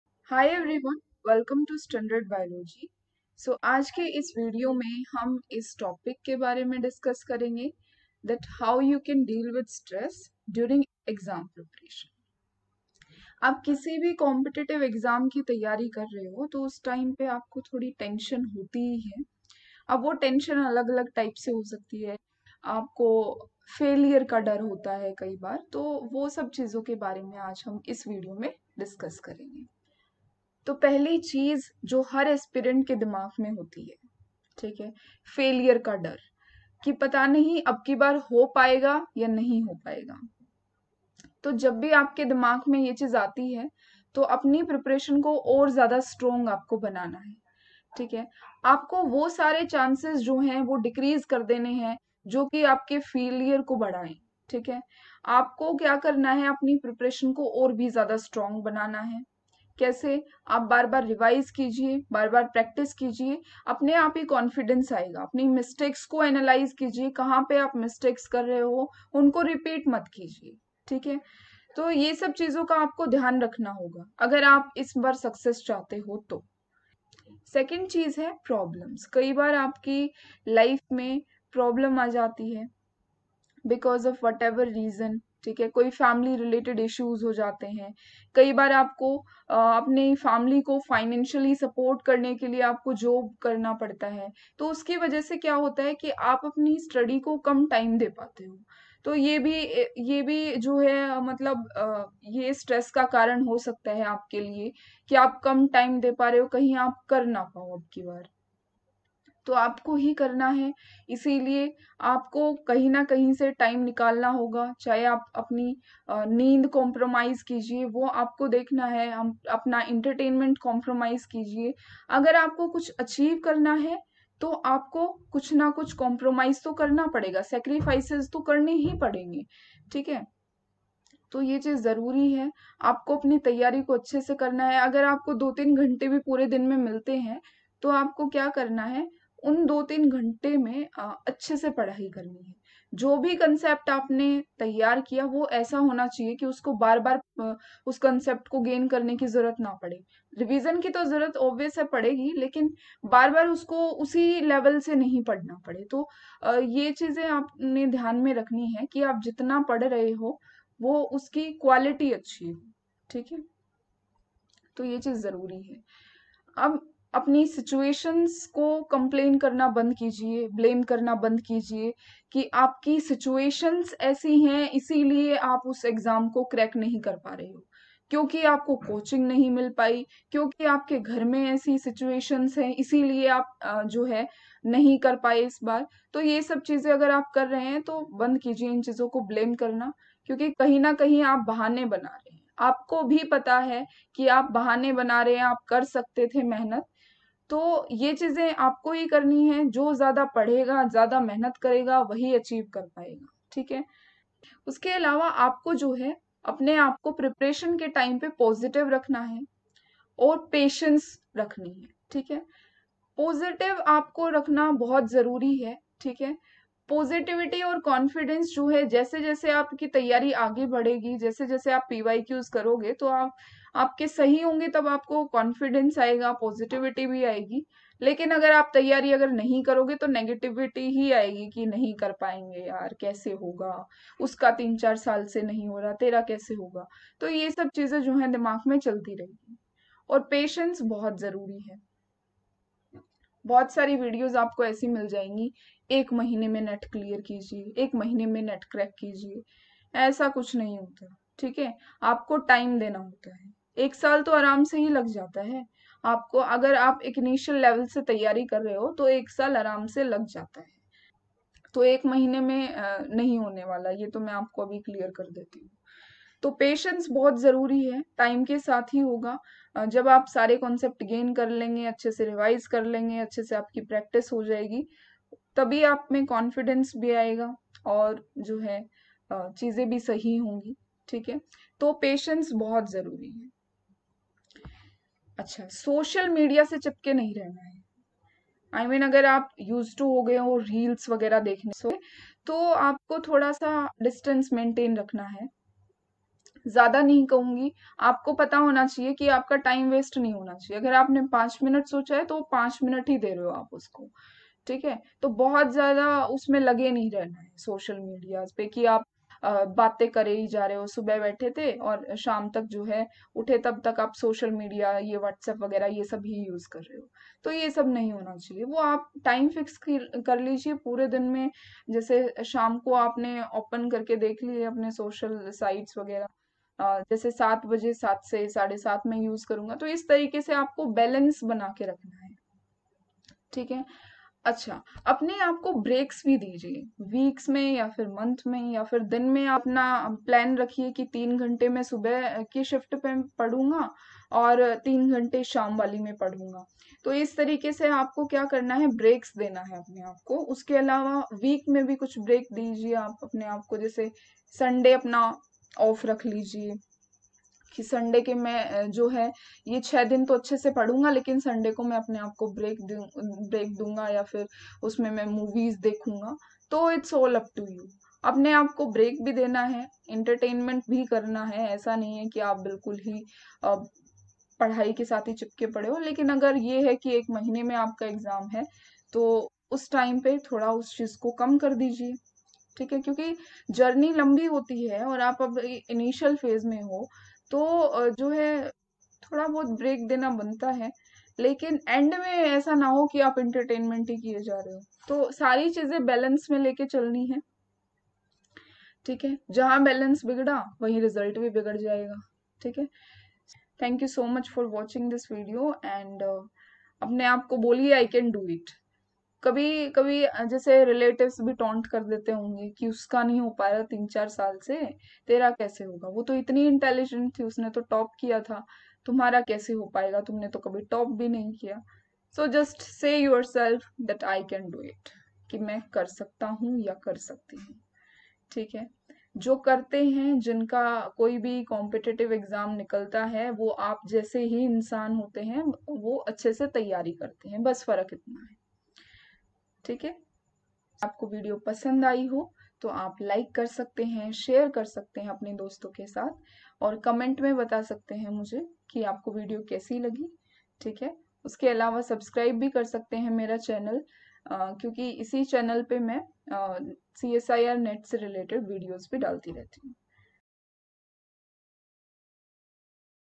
How to deal with stress during competitive exam preparation.
This video explain how you can reduce stress pressure and fear while preparing for any competitive exam

To know strategy to crack CSIR NET jrf you can click on link given below👇